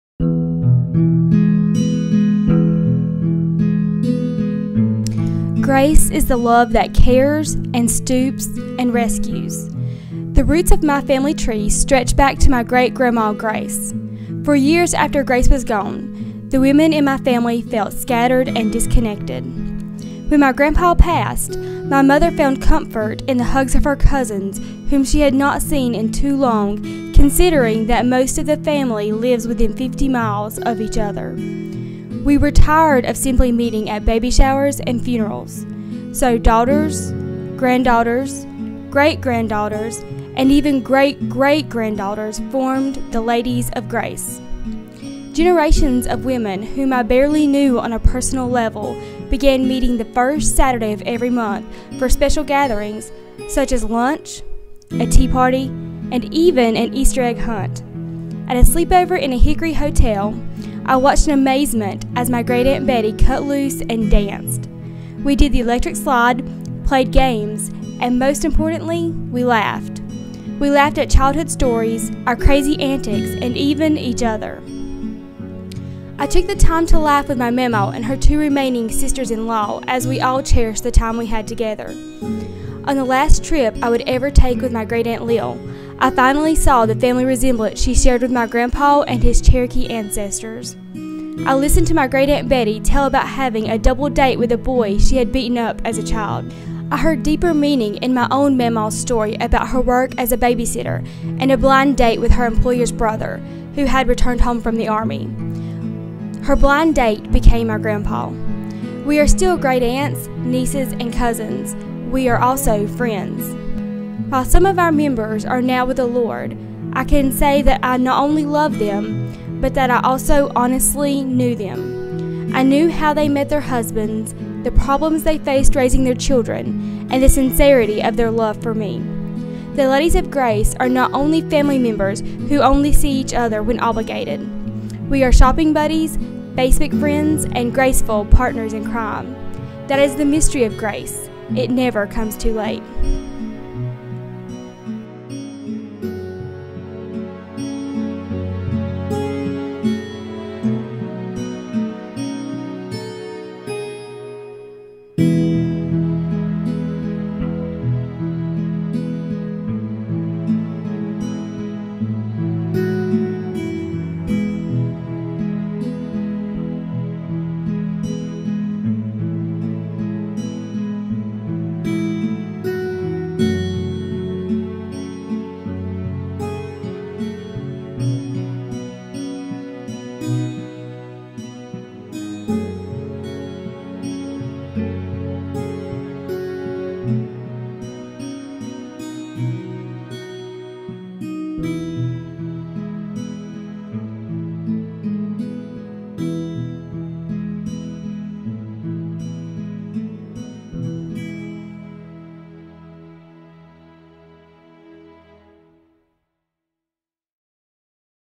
Digital Story: "The Ladies of Grace" by Krysten Buchanan
After the passing of the family matriarch, Great-Grandmother Grace, the scattered women of Krysten Buchanan's family united to form The Ladies of Grace.  They meet monthly to keep their family ties vibrant.  This project was created in the May 2011 narratED Digital Storytelling Workshop.